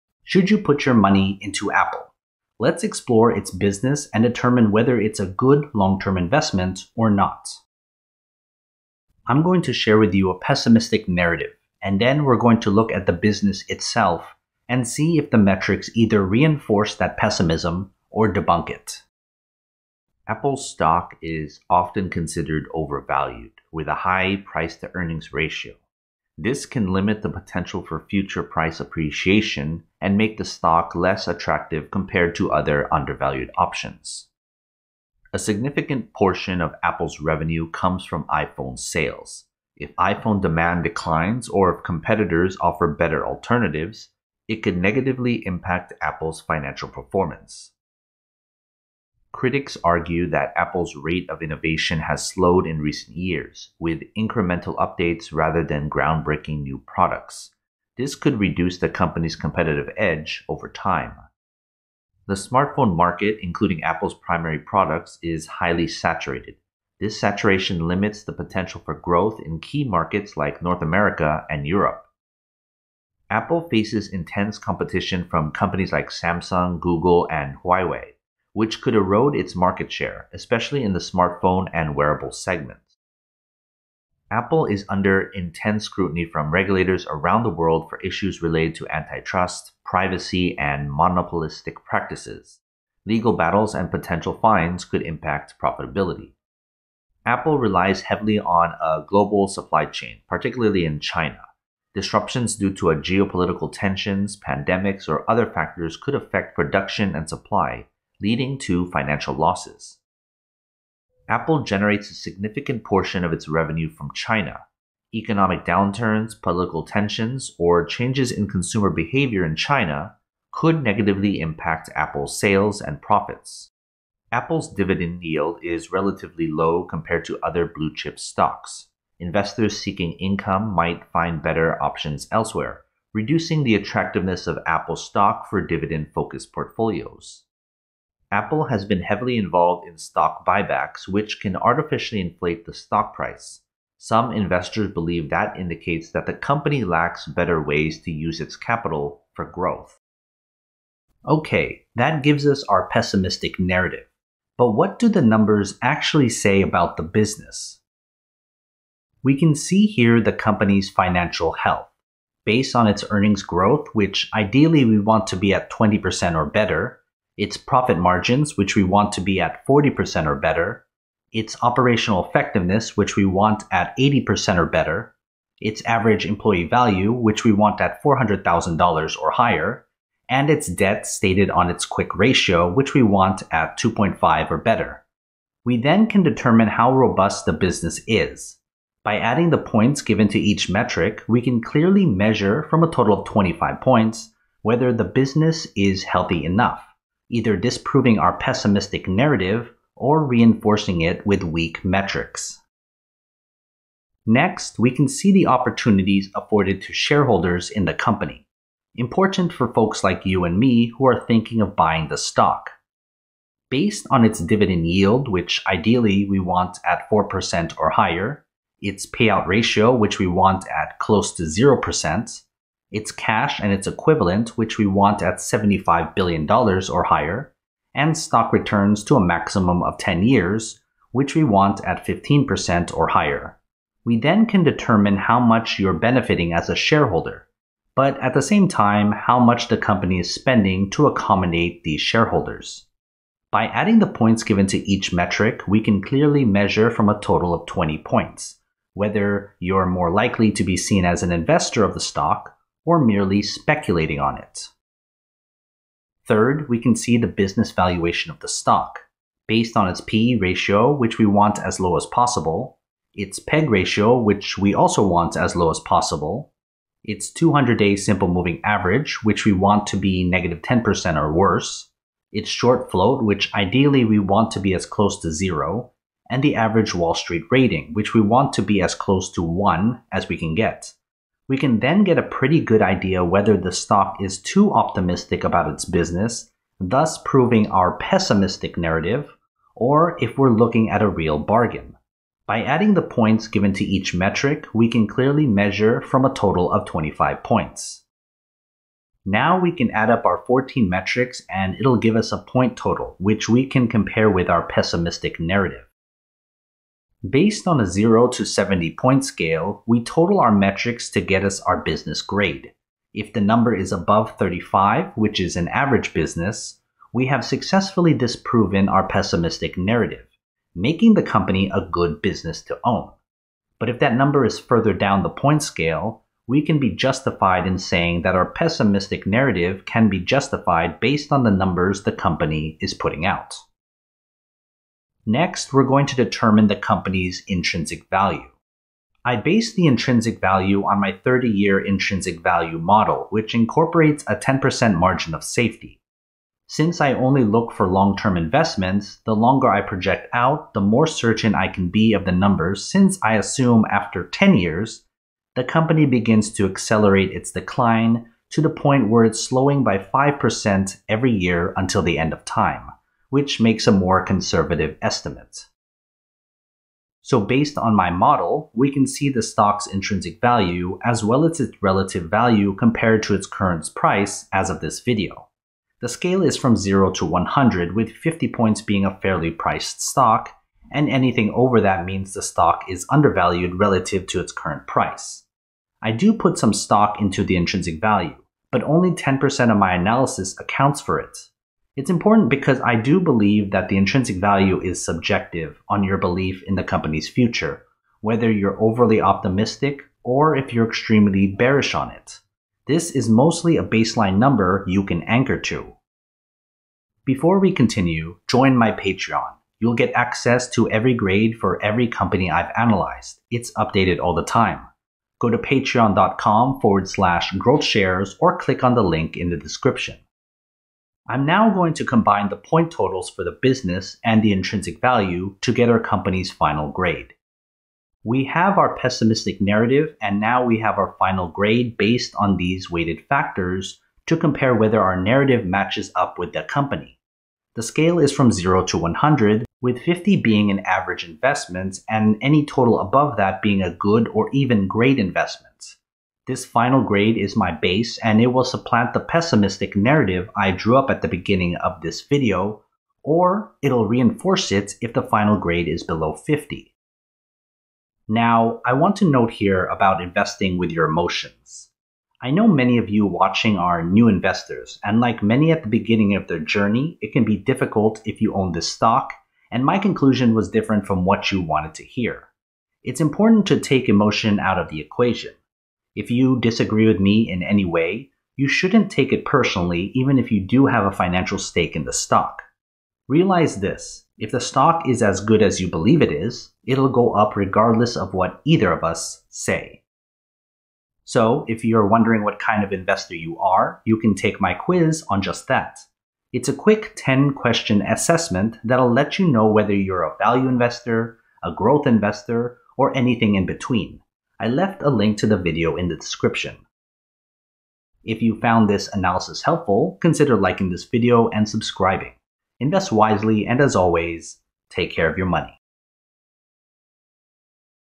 DON'T BUY Apple's Stock (Until You Watch this Analysis)
Is Apple a good long-term investment or should you avoid buying its stock? In this video, I'm going to give you a pessimistic narrative and compare that with its business to see if that pessimism is justified or if we really have a good investment.